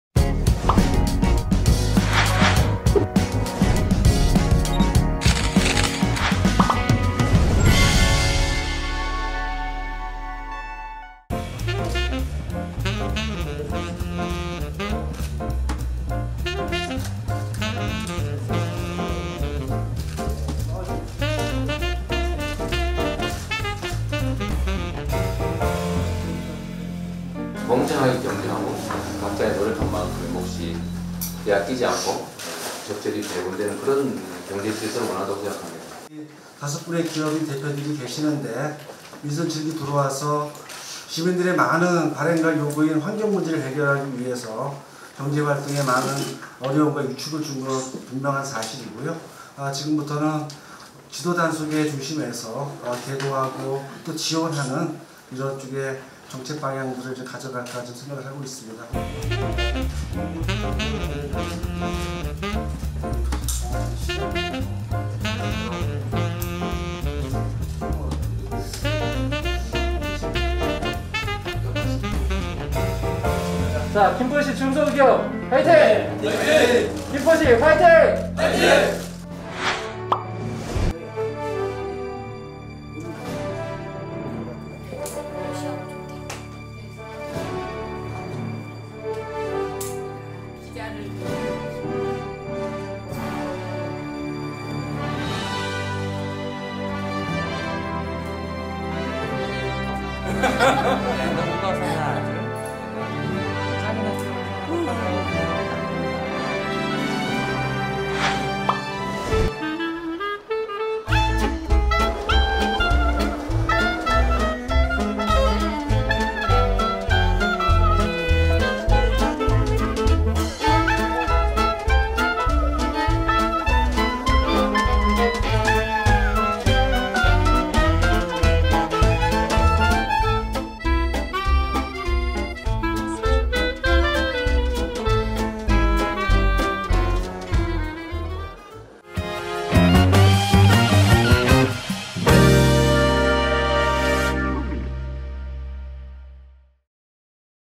[김포시 시정뉴스]2019 11월 첫째주 시정뉴스
-김포시 중소제조업 기업애로 간담회 이재명 경기도 지사 방문
(10.31. 아하정보통신)

-2019 제2회 김포청소년영화제 홍보대사위촉식(11.1. 김포시청 접견실)

-2019 대한민국 우수상품전시회(10.31. 킨텍스)